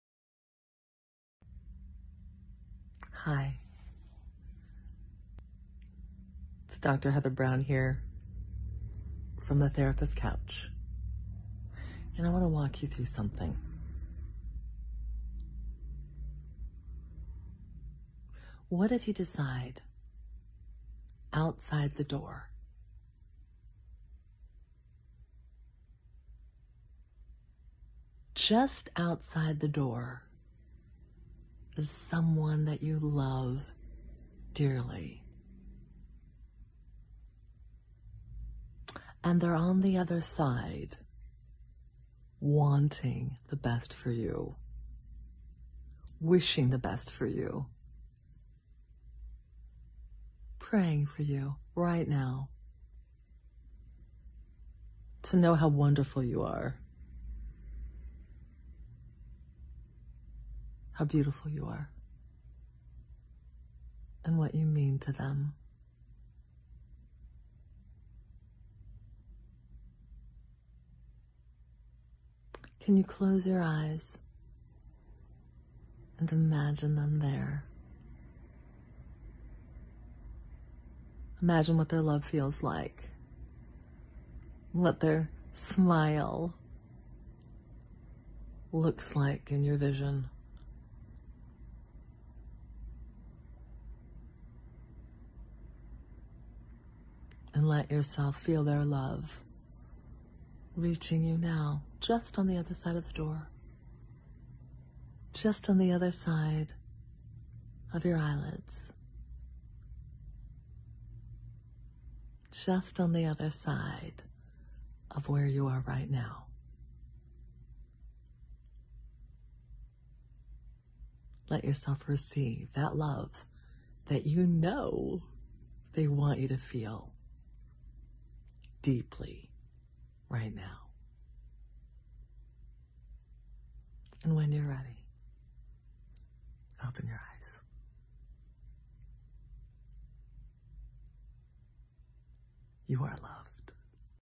The Door Gift. self love
You can change your thought about your life right now.  You can bring yourself love right now.  What if at this very moment you choose more love?